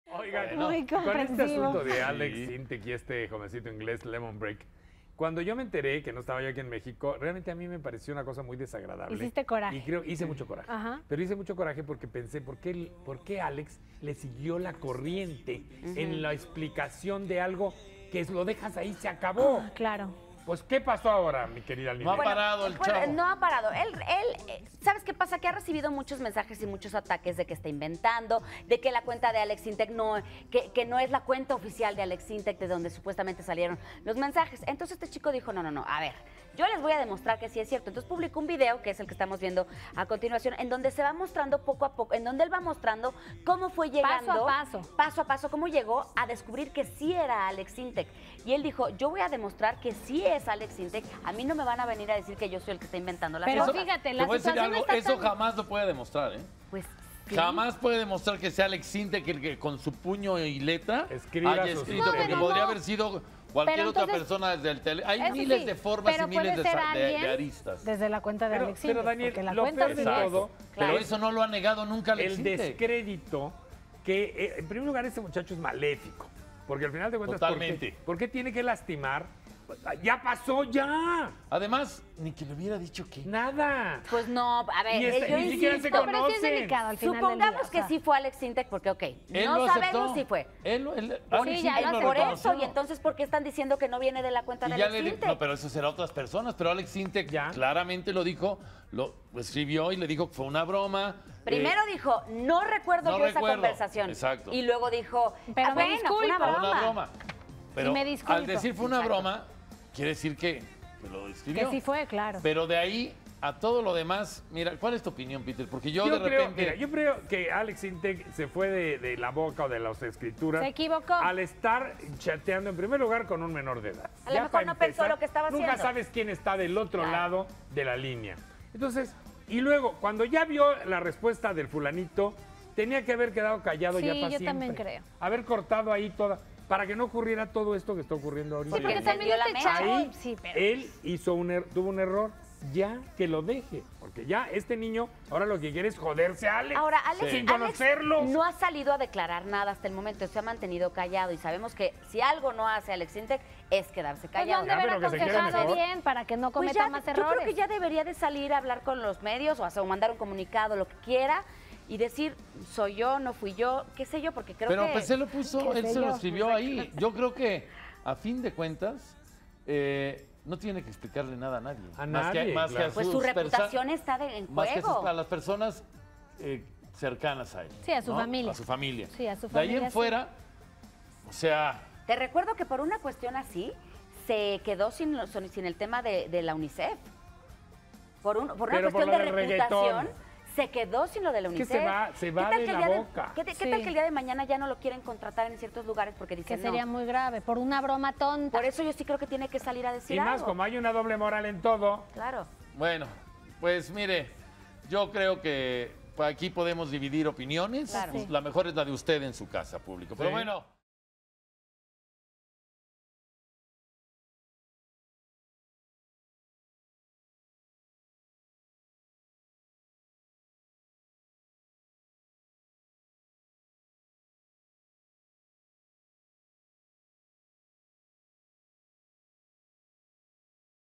¡Volvieron a ventanear a Aleks Syntek en redes sociales!
En Instagram se publicó un video de una supuesta conversación privada entre el chico y el cantante, quien afirma que no van a tratarlo de mentiroso